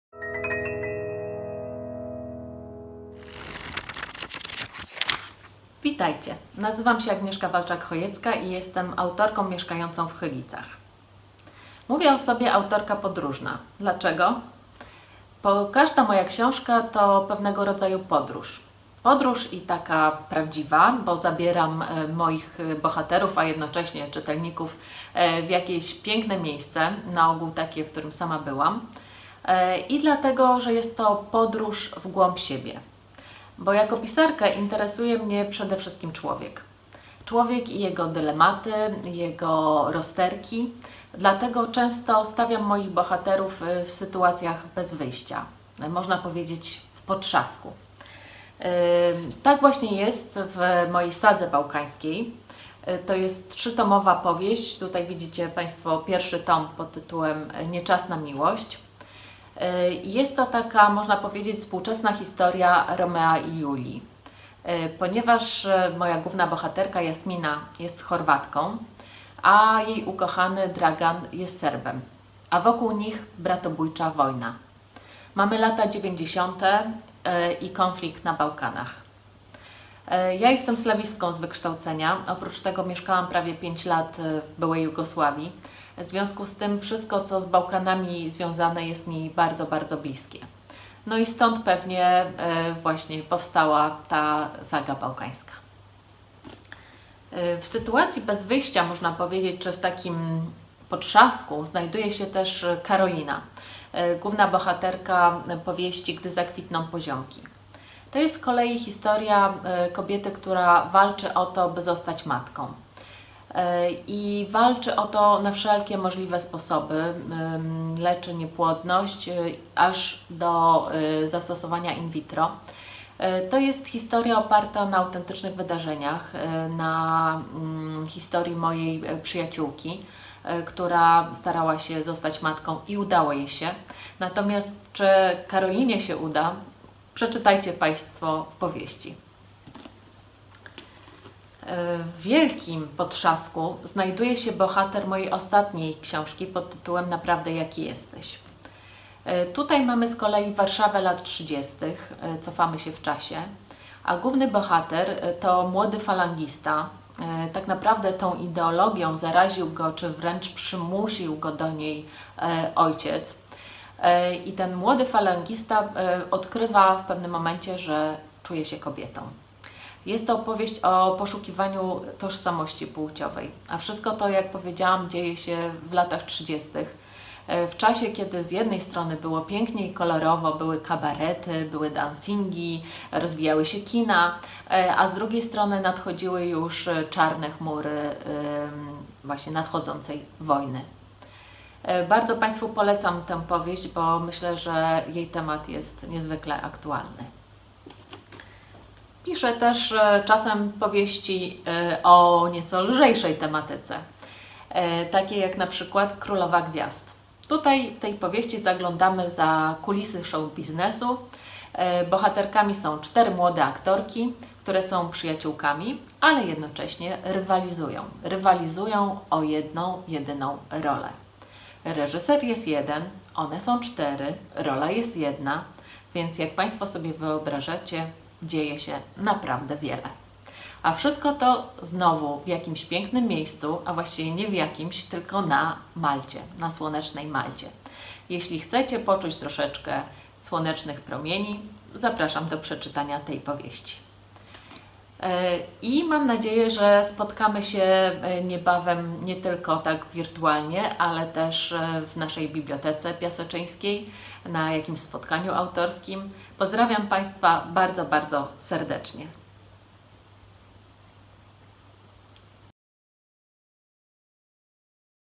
FESTIWAL // Agnieszka Walczak-Chojecka
Autorka powieści obyczajowych goszczących na listach bestsellerów i scenarzystka.
Ukończyła Filologię Słowiańską na UW. Tłumaczyła z języka serbskiego, współpracowała z Radiem Belgrad, III programem Polskiego Radia, czasopismami muzycznymi. Przez wiele lat zarządzała marketingiem i komunikacją w dużych korporacjach. Od 2012 roku zajmuje się zawodowo pisaniem. Owocem tego są jej powieści: Saga Bałkańska: ”Nie czas na miłość”, ”Nie czas na zapomnienie” i „Nie czas na pożegnanie”; „Gdy zakwitną poziomki”, „Dziewczyna z Ajutthai”, „Włoska symfonia” (wszystkie Wydawnictwo Filia) oraz „Królowa Gwiazd” i „Naprawdę jaki jesteś” (Edipresse Książki).

Festiwal organizowany jest w ramach projektu Festiwal Pięknej Książki „Piaseczno, Ludzie, Książki” realizowanego przez Bibliotekę Publiczną w Piasecznie oraz Fundację „Arka” im. Józefa Wilkonia we współpracy z Centrum Kultury w Piasecznie oraz dofinansowanego ze środków Narodowego Centrum Kultury w ramach programu Kultura – Interwencje 2020.